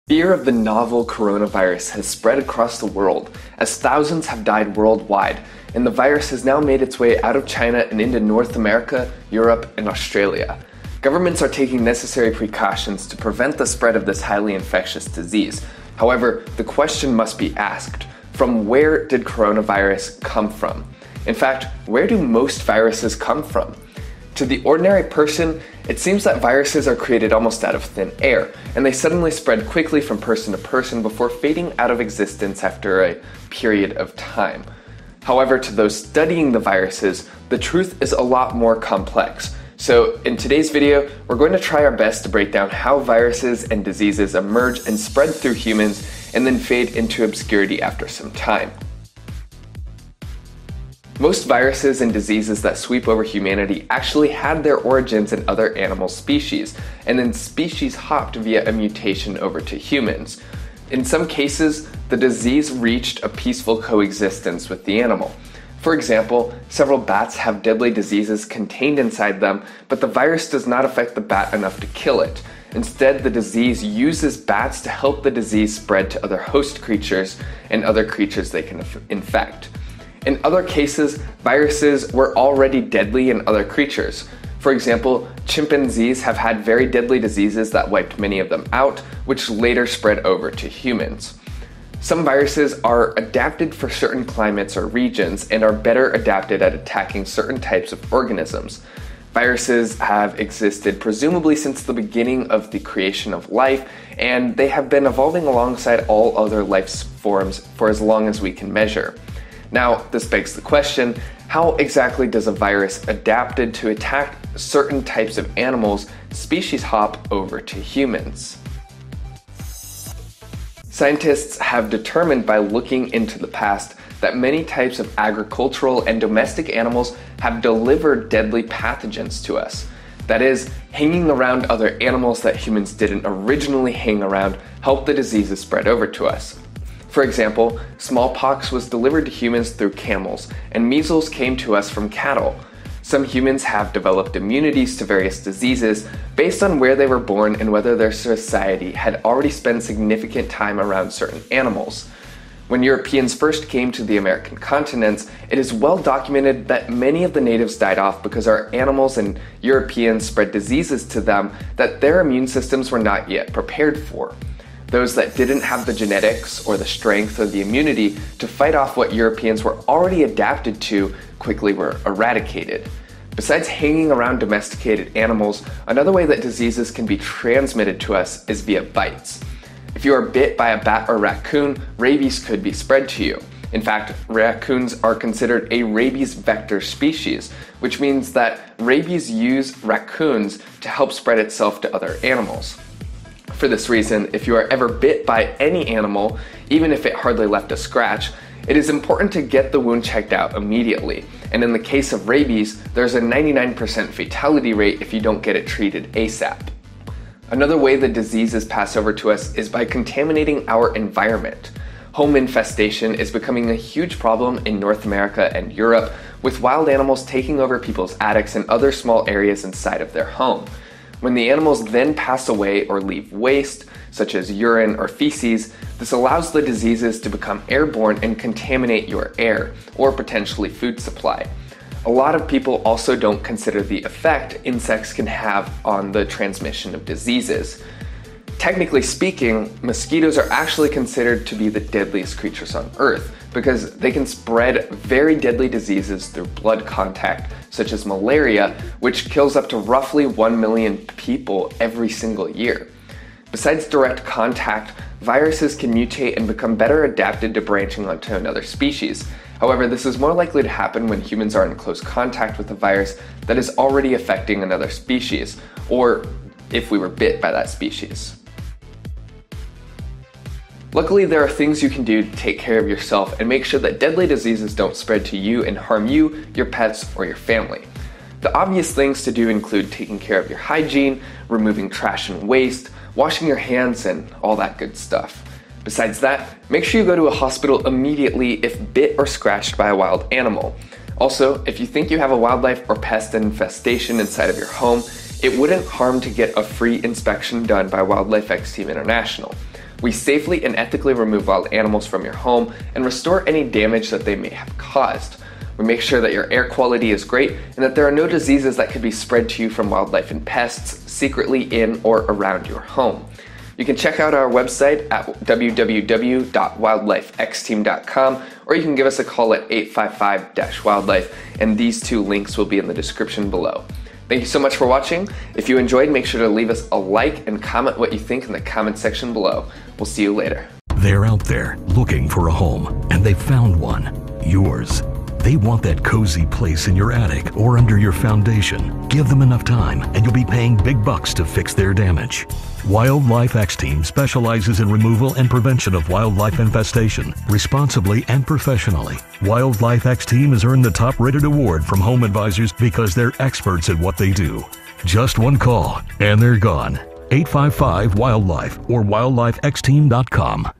Where Do Viruses Come From?
Where did the Novel Coronavirus come from? In fact, where do all viruses & diseases originate from?

A lot of people don't understand that viruses often originate in animals, and they also don't understand how viruses can mutate to species-hop over to humans.

In today's video we go deep on how viruses originate, and then later fade out of obscurity, due to mutations, and other fascinating methods.

Got wildlife or pest problem? Prevent the spread of deadly diseases by calling us now:
855-WILDLIFE